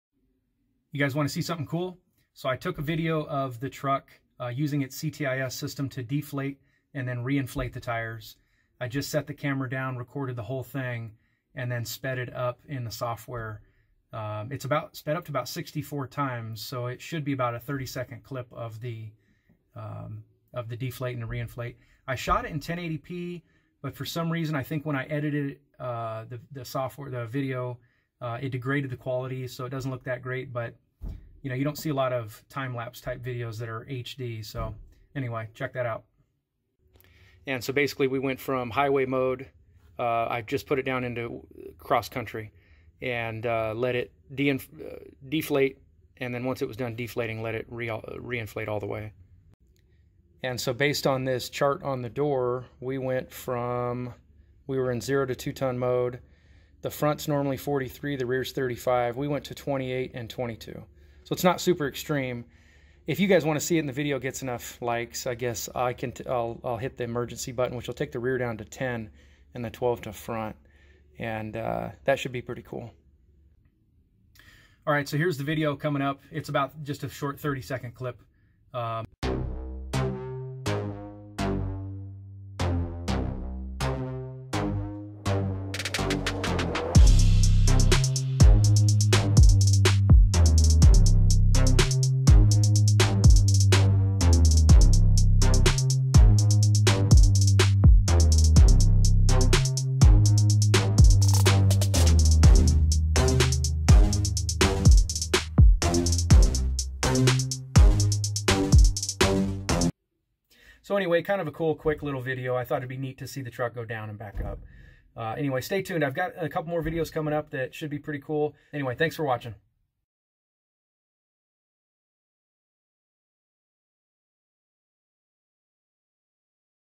CTIS Central Tire Inflation System Oshkosh MTVR 6x6 Military Truck
Time lapse of tires deflating and inflating